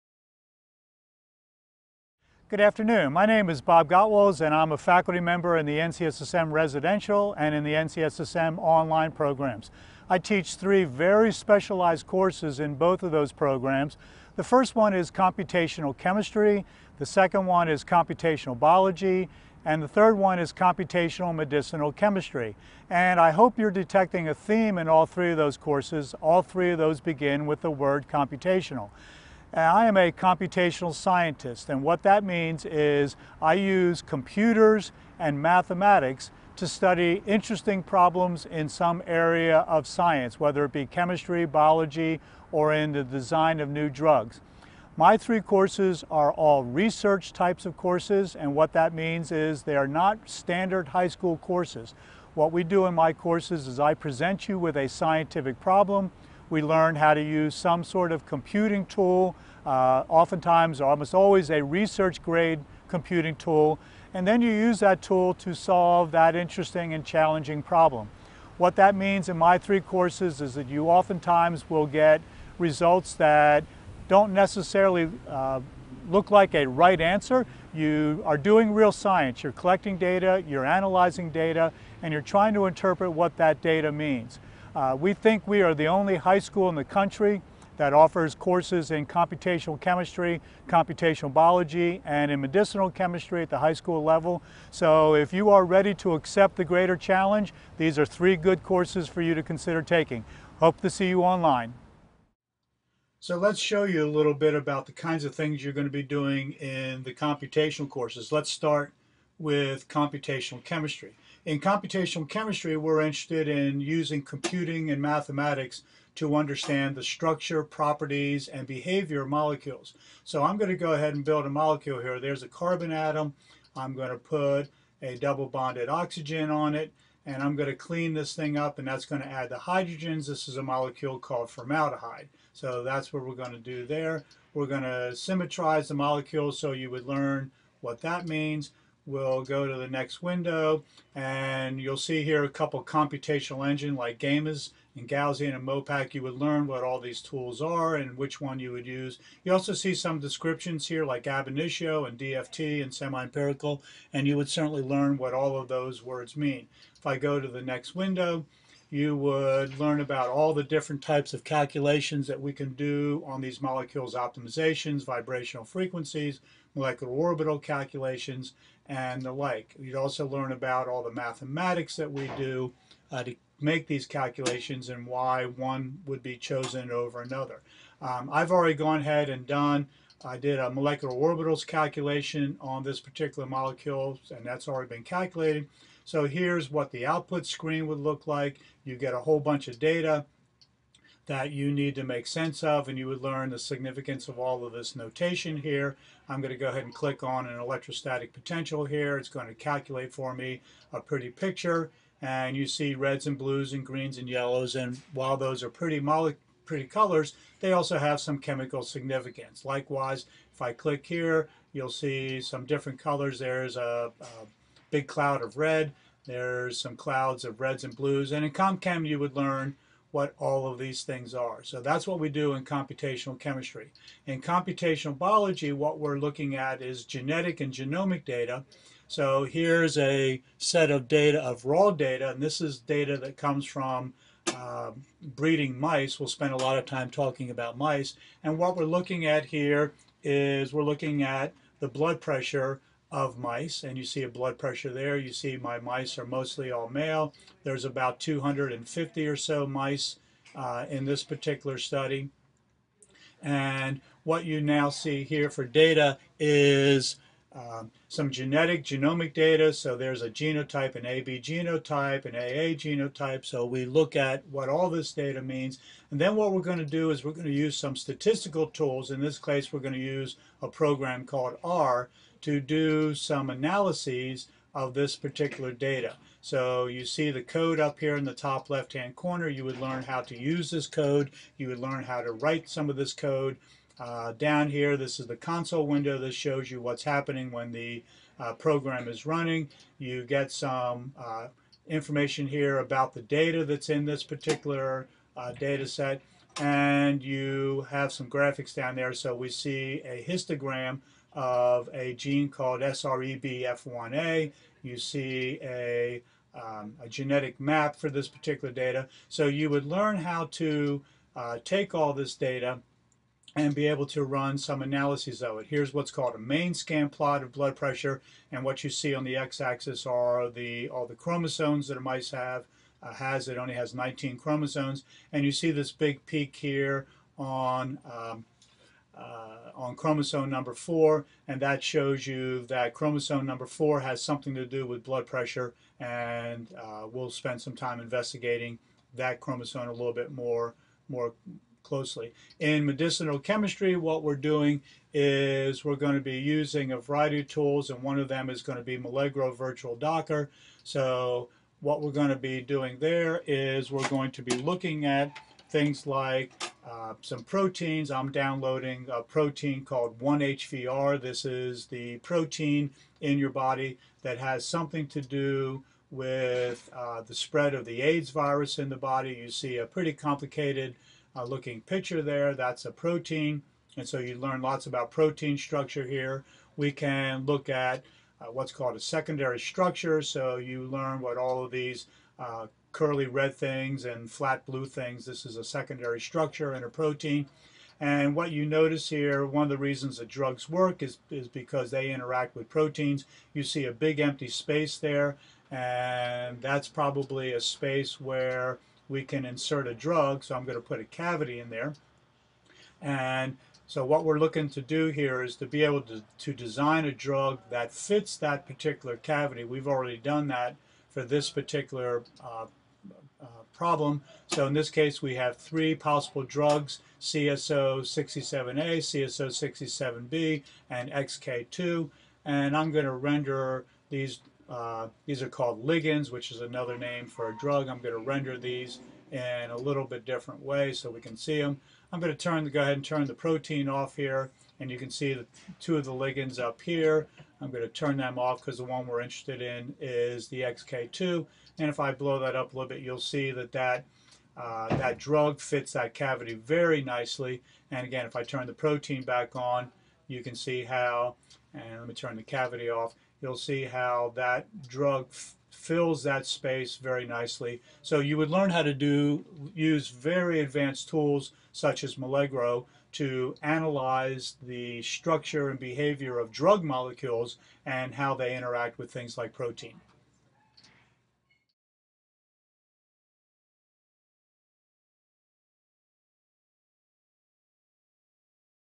[CS]계산과학이란?
- 제목 : Introduction to Computational Sciences

- 내용 : 계산과학이란 학문에 대해 소개하는 영상이다. Bob Gotwals 는 계산과학이란 컴퓨터와 수학을 가지고 과학분야의 흥미로운 문제를 해결하는 학문이라 소개한다. 그리고 나서 계산과학을 가장 잘 보여줄 수 있는 세가지 프로그램을 소개하는데 첫 번째는 화학에 관련된 프로그램으로 분자의 구조를 쉽게 이해하며 공부할 수 있는 프로그램이다. 직접 원하는 원자를 생성한 뒤 나머지 원자들과 이어 붙임으로써 원하는 분자를 만들어 볼 수 있으며 그 분자의 전기음성도나 등등을 프로그램이 계산한 뒤 이미지로 나타내 줄 수도 있다. 두 번째는 생명과학과 관련된 프로그램으로 250마리의 쥐에대한 혈압과 유전자를 조사하여 입력하면 체계적으로 분석하여 특정 유전자에 대한 히스토그램이나 유전자 지도를 그려주고, 혈압에 대한 그래프도 나타내준다. 세 번째는 단백질의 구조를 나타내주는 프로그램으로써 구체적으로 나타낼 수도있고 간단하게 색으로 나타낼 수도 있으며 구조중에 원하는 부분만 볼 수도 있다. 이 프로그램으로 약물에 대한 단백질의 구조를 배울 수 있다고 한다.

- 결론 : 정보과학이 컴퓨터 기반의 창의적 문제해결방법을 탐구하는 학문이라면 정보과학의 응용분야는 계산과학일 것이다. 이 동영상에서 보았던 것과 같이 컴퓨터의 뛰어난 처리능력과 기억능력을 과학의 여러가지 분야에 접목시킨다면 분자구조처럼 실세계에서 복잡한 구조를 나타내기 어렵거나 유전자 지도와 같이 분석하기 위해서는 시간이 오래걸리는 것들이 가능해 질것이며 그로인해 접목된 분야도 발전될것이다. 또한 이 영상을 보며 앞으로의 과학분야가 컴퓨터에 대한 의존성이 커질것이라 예상했다.